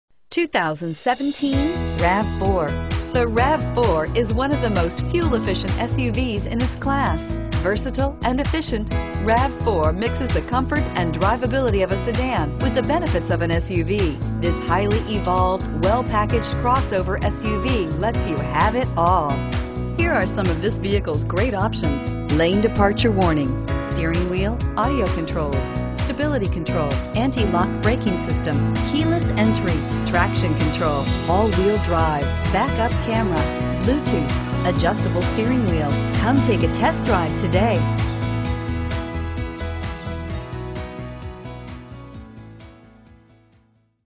2017 Toyota RAV4 Lee's Summit MO 553181
2017 Toyota RAV4 LE

Adams Toyota
501 NE Colbern Road

You'll love this 2017 Toyota RAV4. This is a car you'll want to take home. With  miles, it features Automatic transmission and an exterior color of Silver Sky Metallic.